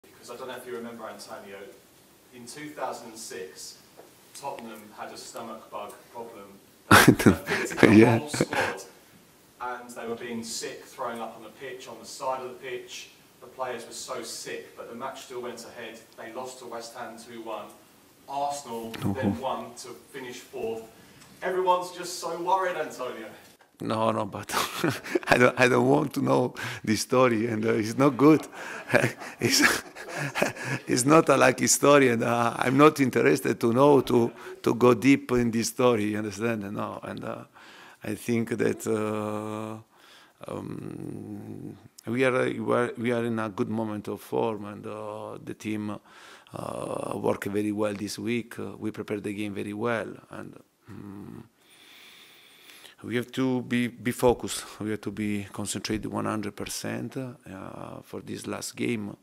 I don't want to know the 2006 Lasagnegate story! Antonio Conte 🤣
Antonio Conte: on Lasagnegate in 2006
I don't want to know this story. I'm not interested to go deeper into this story. (Laughs) I think that we are in a good moment of form. We have worked well this week and prepared very well. We have to be concentrated.